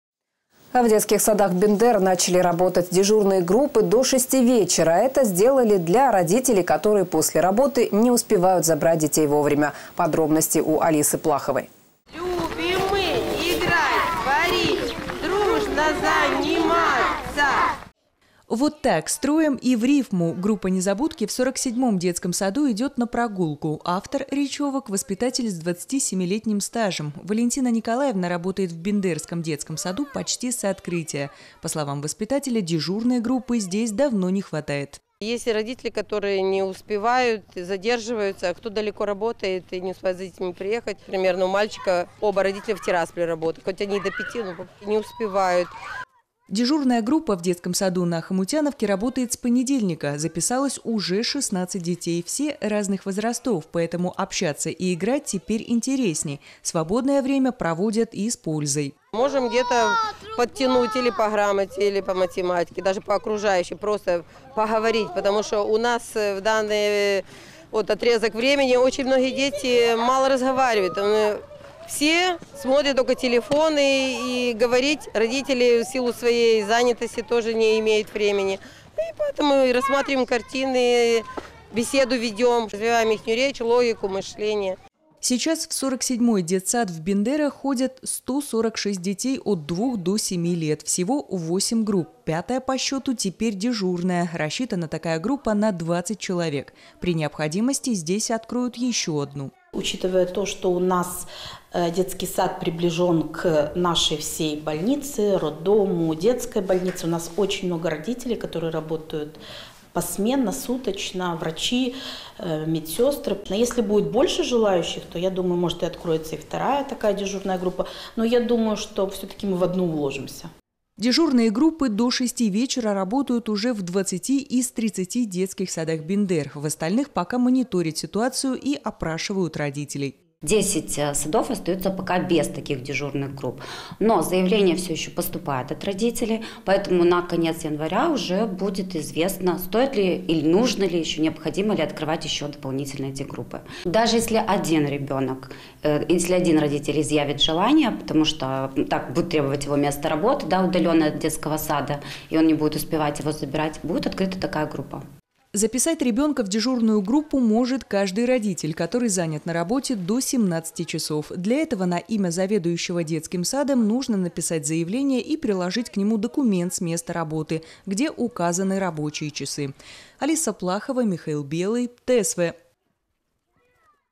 В детсадах Бендер открылись продлённые группы
В детских садах Бендер начали работать дежурные группы до 6 вечера. Это сделали для родителей, которые после работы не успевают забрать детей вовремя.